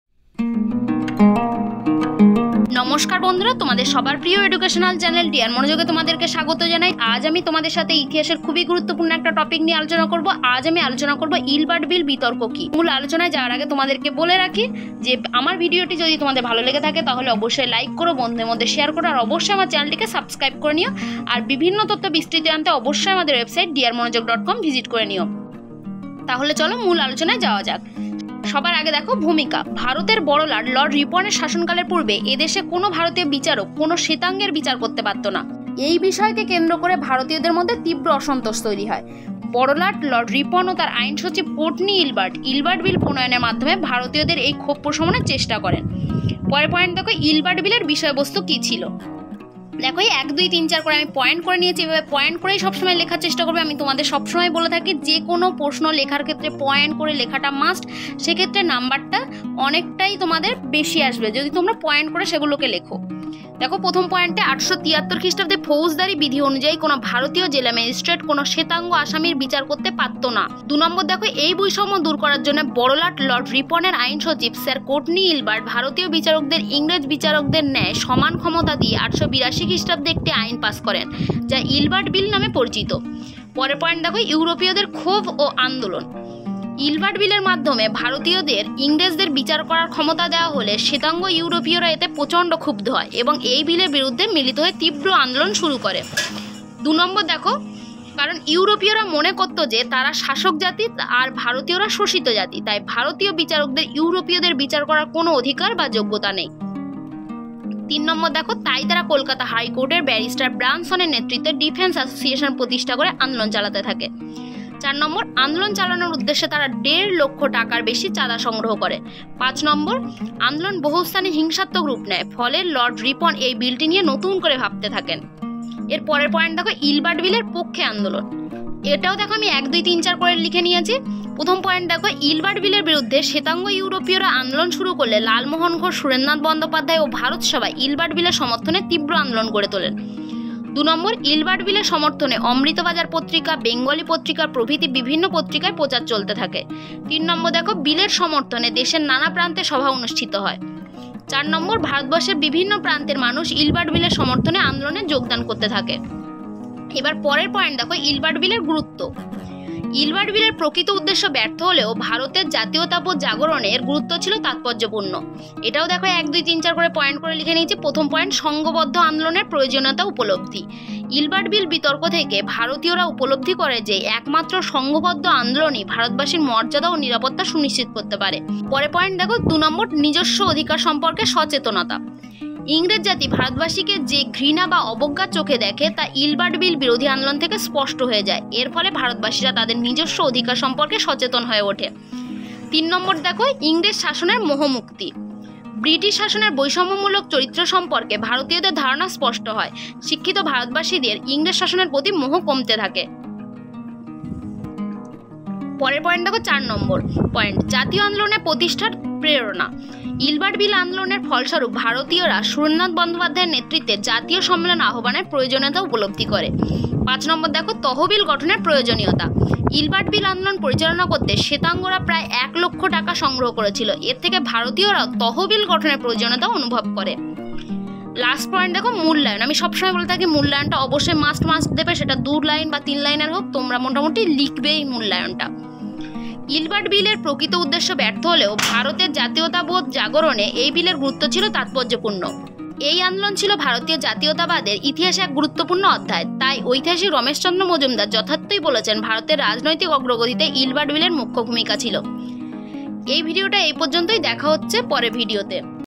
ইলবার্ট বিল বিতর্ক কি || নোটস সহ সম্পূর্ণ পয়েন্টভিত্তিক আলোচনা ।। ডি আর মনোযোগ
ফৌজদারী বিধি অনুযায়ী কোনো ভারতীয় জেলা ম্যাজিস্ট্রেট কোনো শ্বেতাঙ্গ আসামীর বিচার করতে পারতো না । এই বৈষম্য দূর করার জন্য বড়োলাট লর্ড রিপণের আইন সচিব স্যার ইলবার্ট ভারতীয় বিচারকদের ইংরেজ বিচারকদের নায় সমান ক্ষমতা দিয়ে ১৮৮২ খ্রিস্টাব্দে একটি আইন পাশ করেন, যা ইলবার্ট বিল নামে পরিচিত।